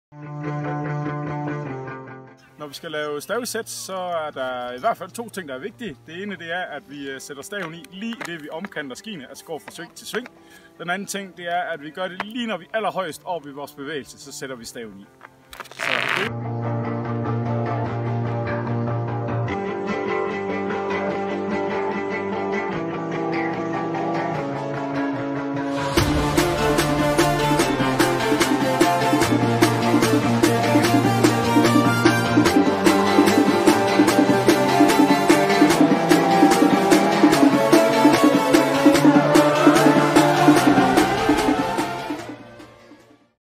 Sådan bruger du skistavene rigtigt
Jacob Struve, lærer på Ski- og snowboardlinjen på Idrætshøjskolen i Aarhus viser dig hvordan du laver stavisæt